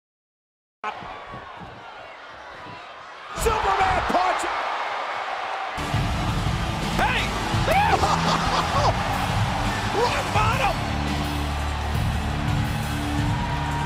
THE ROCK BOTTOM AT WRESTLEMANIA!
The Rock hits a rock bottom on Roman Reigns at Wrestlemania! Here's hoping they do this match at wrestlemania this year! It would be a banger match up!